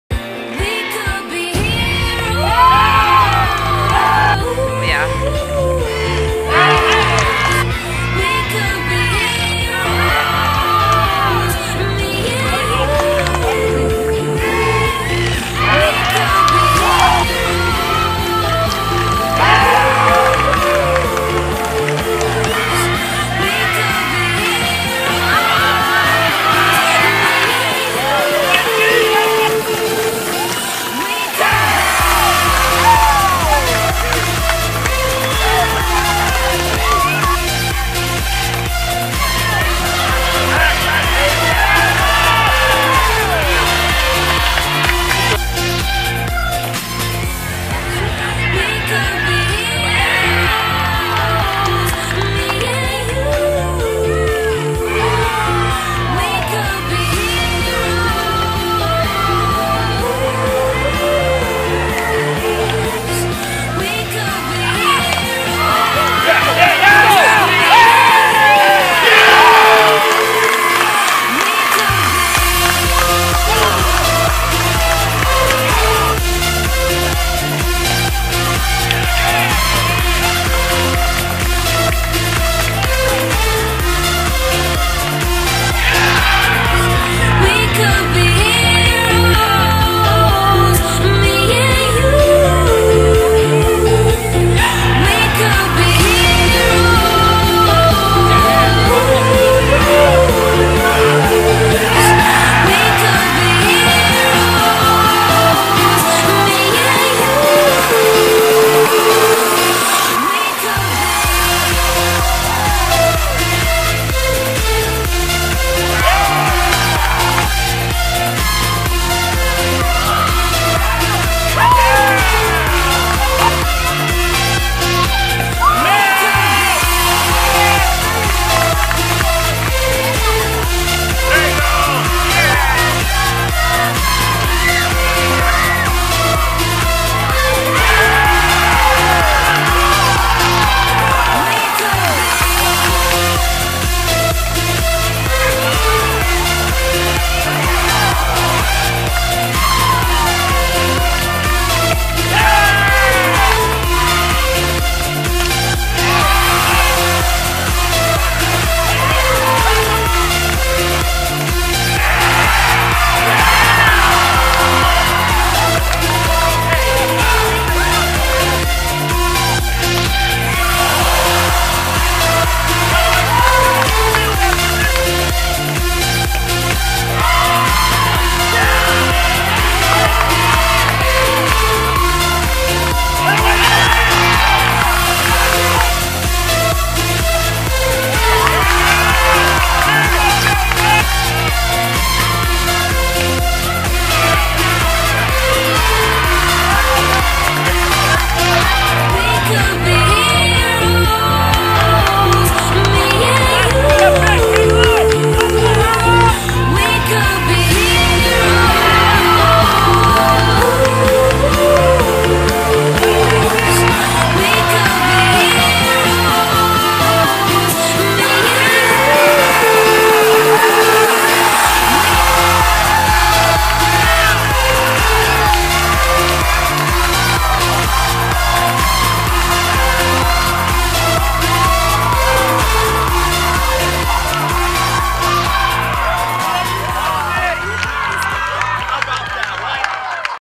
Minnesota Battles in Regional Championship
Here's are the final highlights from the 2015 campaign. Check out some of our favorite moments from Sunday's regional championship versus Arizona. The Gophers mounted two impressive seventh inning comes backs including a walk-off grand slam from Paige Palkovich to push the decisive winner-take-all second game. Minnesota ultimately came up just short in extra innings of the game two but battled until the very end.